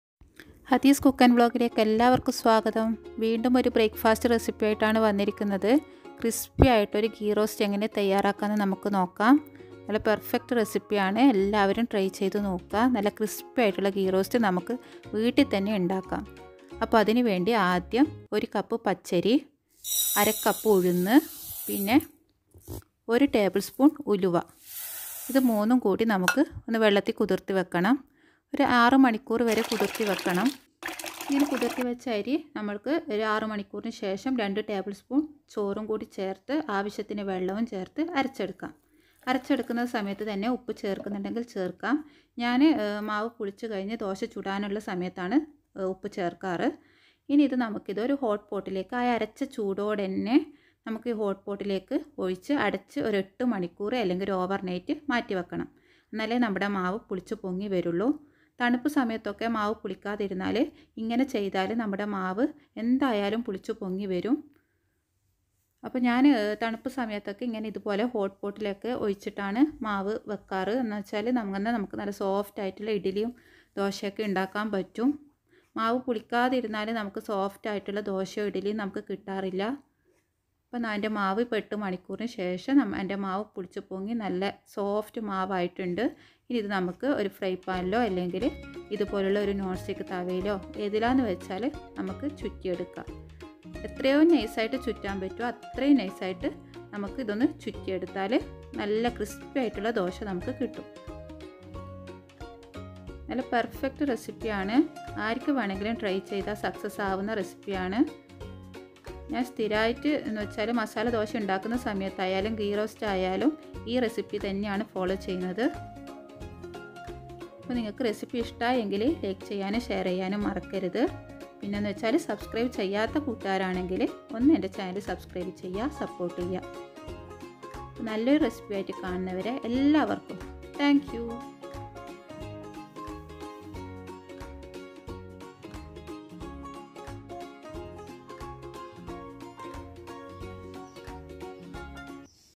How to make crispy dosa / ghee roast malayalam recipe / നെയ് റോസ്റ്റ് റെസിപ്പി / நெய் ரோஸ்ட் தோசை
solved query :
restaurant style gheeroast
indian coffee house ghee roast
easy malyalam breakfast recipe
healthy break fast recipe
ney roast recipe